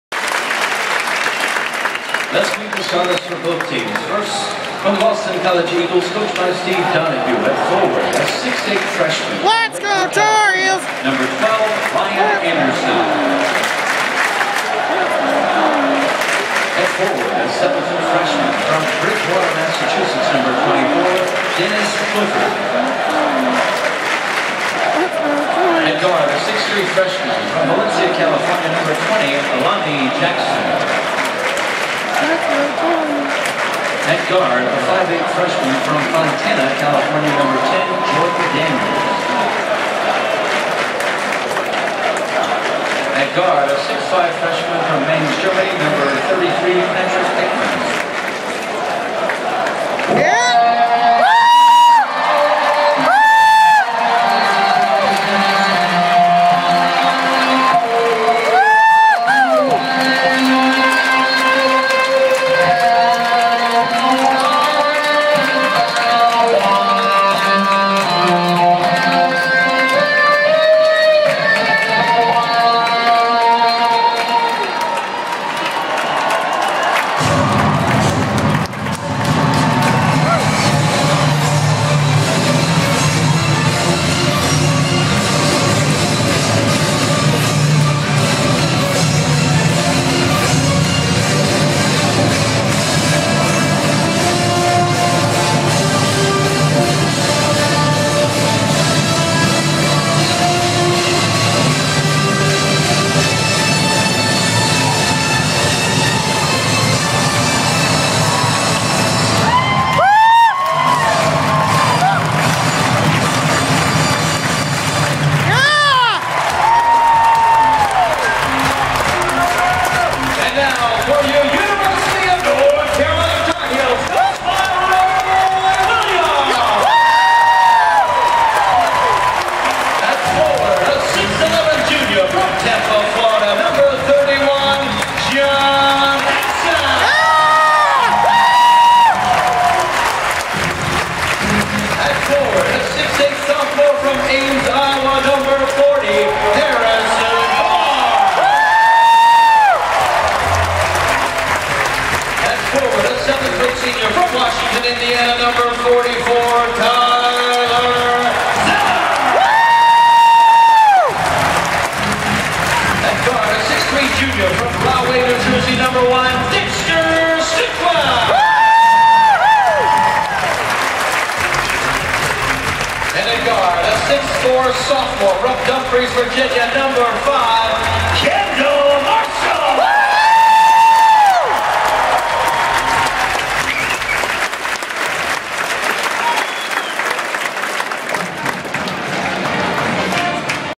Carolina basketball introduction 2012
The Tar Heels are introduced for their first conference game, against Boston College